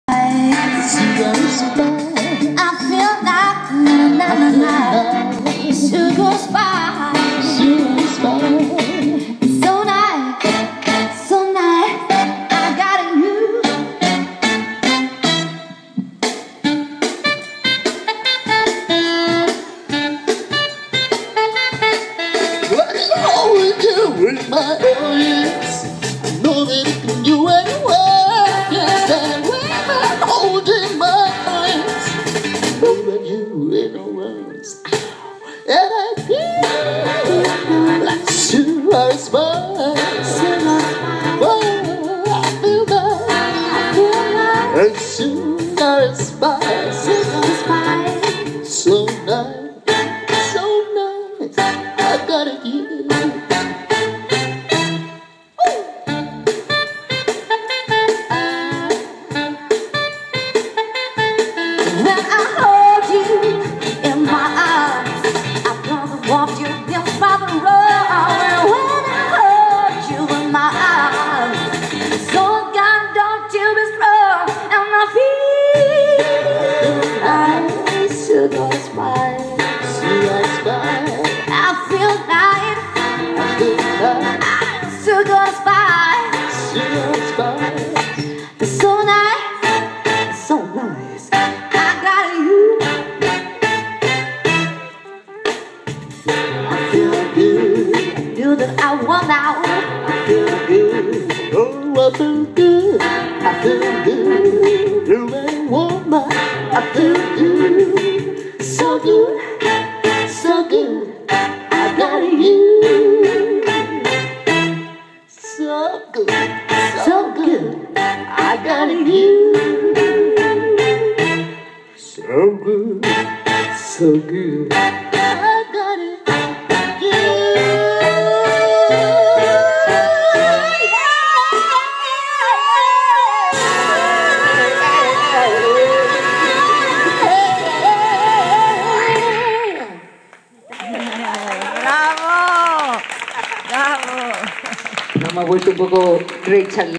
Hortensias y Aspidistras - SonSoul
Maravillosas Eli Hernandez y María Romero, componentes del trio granadino Son Soul cantan en directo en la Terraza-Jardín Hortensias y Aspidistras (Alfacar-Granada)